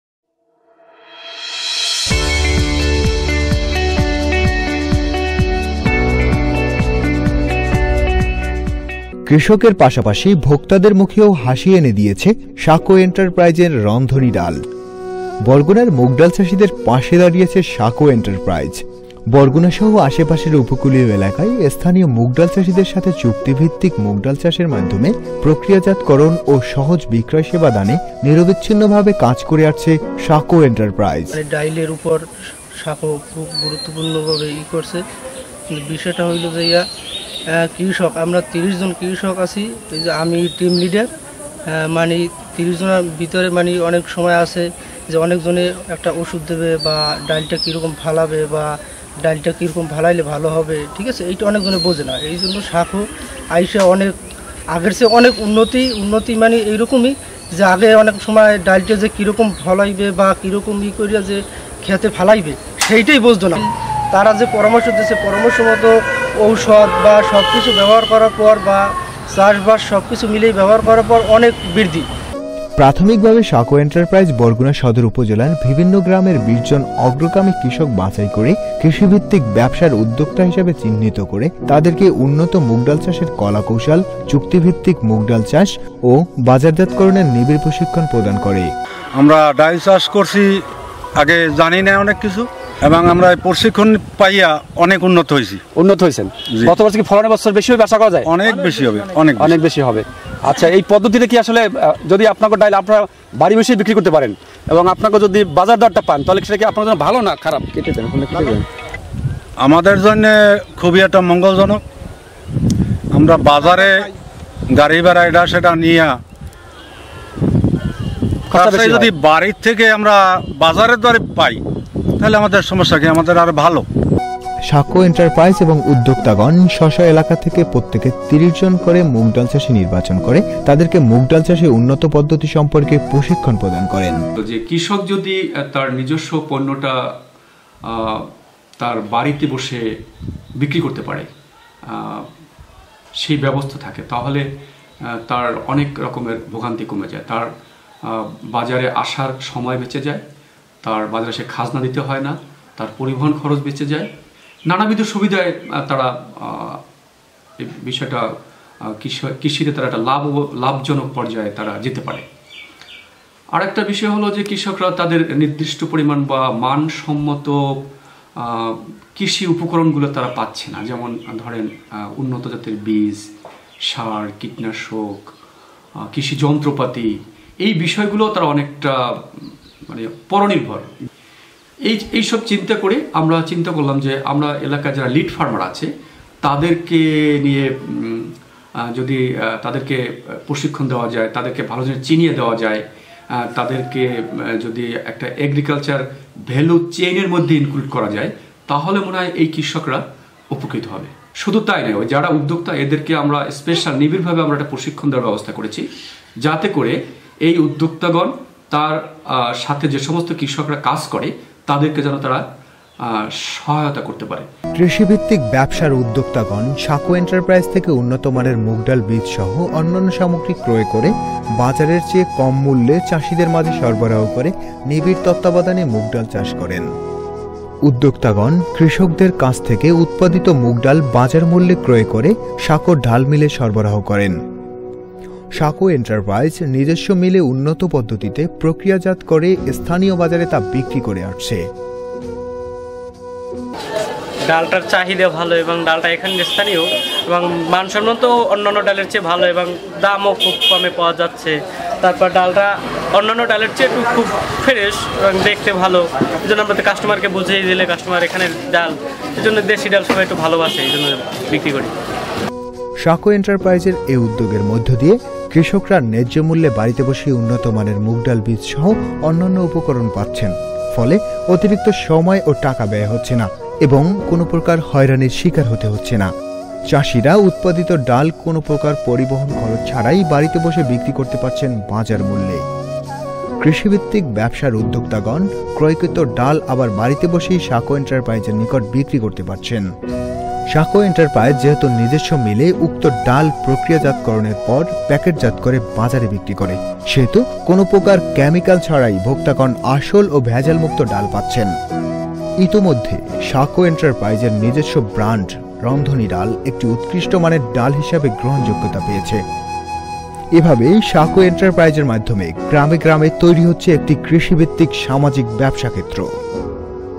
Documentary on Mung Bean by Saco Enterprise
Saco Enterprise has been supporting the mung bean farmers in Barguna district with the support of Feed the Future Bangladesh Rice and Diversified Crops Activity. This process documentary tells the story of helping farmers to produce better crops.